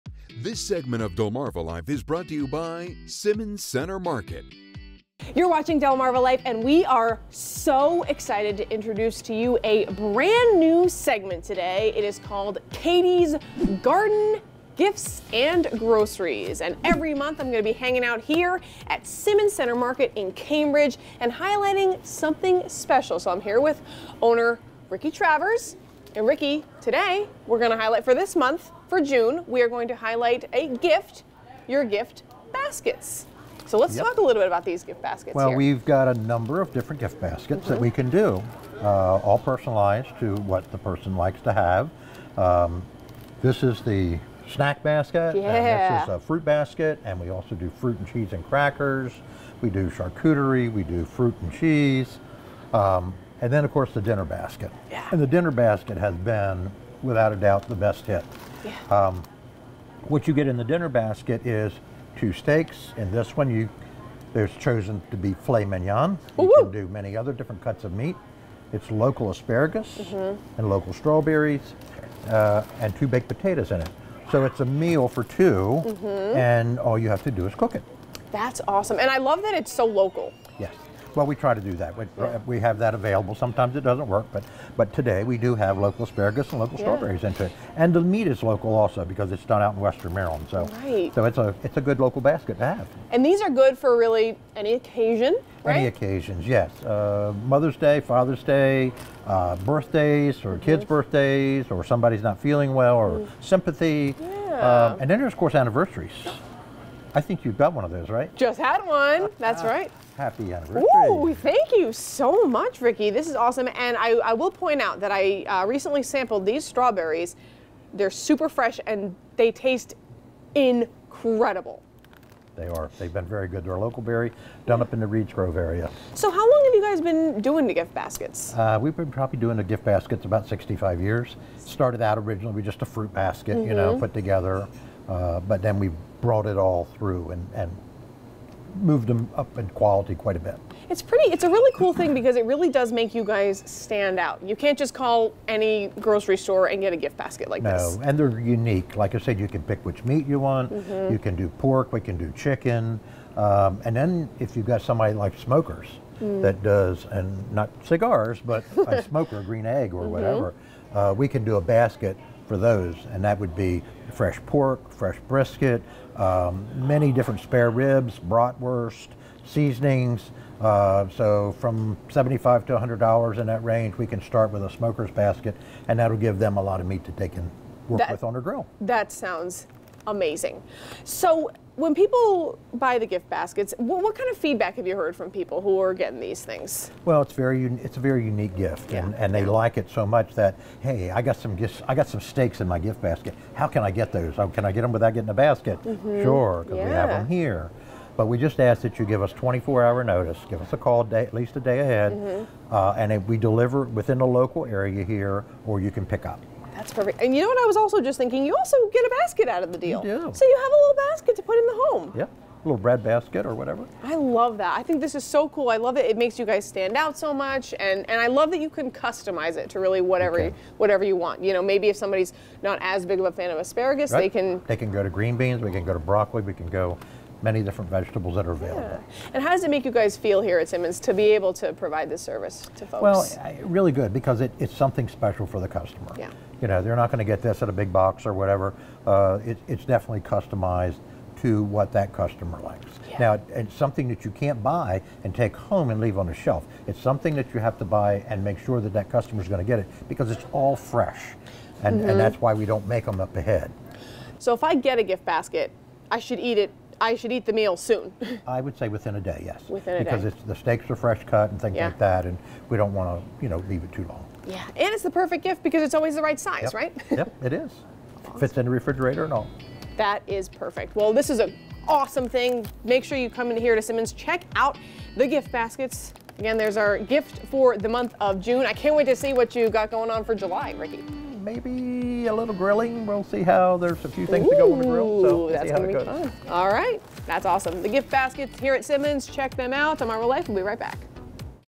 Simmons Gift Baskets | Katie's Garden, Gifts, & Groceries
We're kicking off Katie's Garden, Gifts & Groceries series with Simmons Center Market checking out their delicious gift baskets.